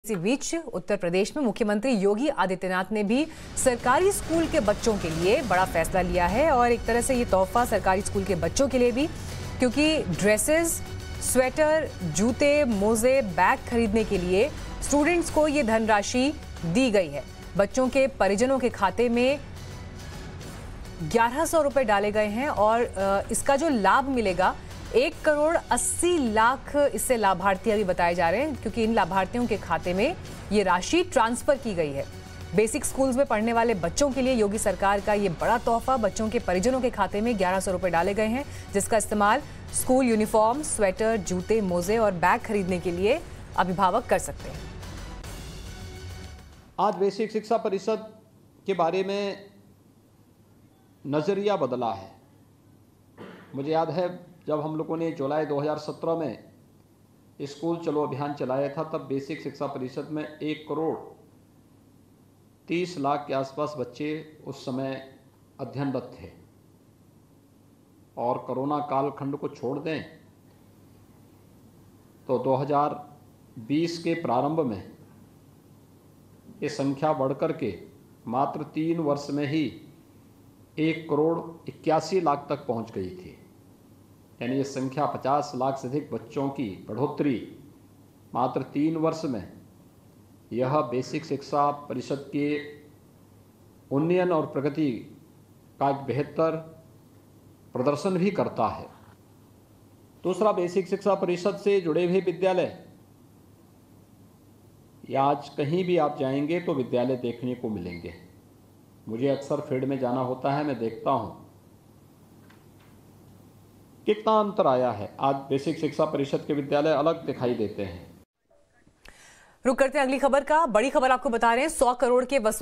CM Yogi का बड़ा फैसला Primary Schools के बच्चों को देगी 11-11 सौ रुपये, जानें- पूरी योजना
Uttar Pradesh सरकार ने लिया बड़ा फैसला, बेसिक स्कूल में पढ़ने वाले हर बच्चे के स्वजन के खाते में 11 सौ रुपये की राशि भेजी, ड्रेस, जूते खरीदने में प्रयोग की जाएगी यह राशि.